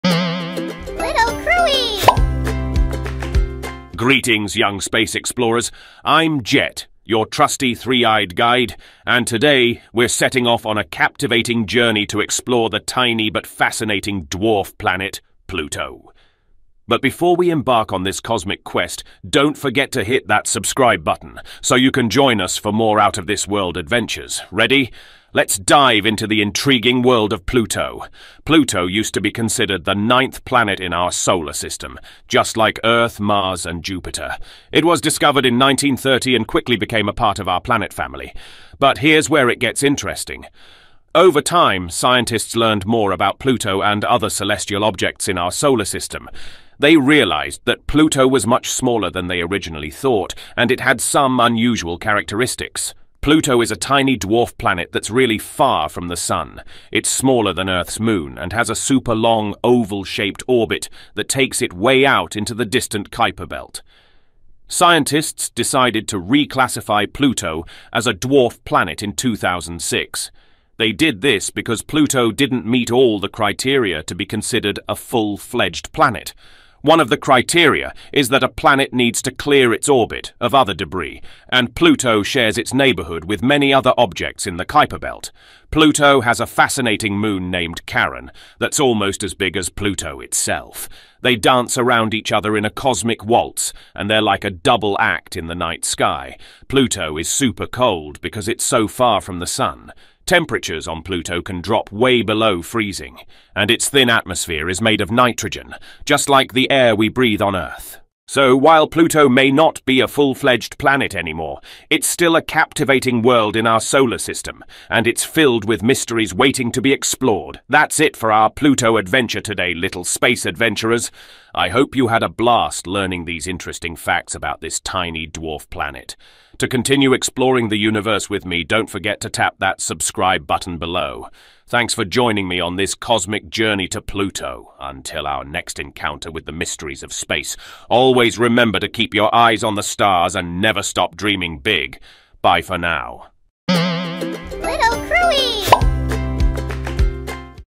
Pluto: The Tiny World with a Big Story for Kids
🚀 Join us on an extraordinary adventure to the dwarf planet Pluto! In this exciting and educational video created especially for kids, we'll uncover the mysteries of Pluto, its unique characteristics, and the incredible story behind its place in our solar system.

❄️ Explore Pluto's icy surface, learn about its heart-shaped feature, and dive into its intriguing history. Discover why Pluto was reclassified as a dwarf planet and what makes it so different from the other objects in the Kuiper Belt.

🌟 This video is filled with captivating visuals, fun facts, and easy-to-understand explanations, making it the perfect learning experience for young space enthusiasts. Join us on this cosmic journey to explore the wonders of Pluto! 🛰️🌠